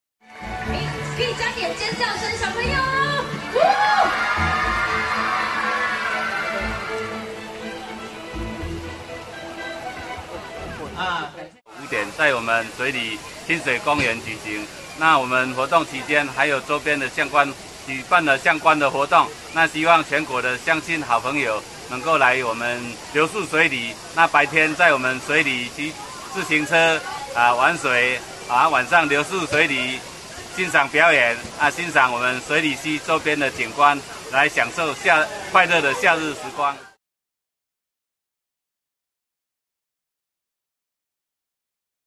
這個縣不靠海…玩水節小農市集、洋妞樂一「夏」
南投暑假玩水盛事，一年一度水里玩水節，7月6日將在水里親水公園盛大登場，今年活動時間提早到7月第1週就開跑，連續6個週末六、日都有系列活動與玩水節目，鄉公所希望在不靠海的南投縣，提供民眾一個安全的玩水場地，今還有金髮碧眼的模特兒搶先體驗玩水設施，大讚沁涼好玩，活動至8月11日止。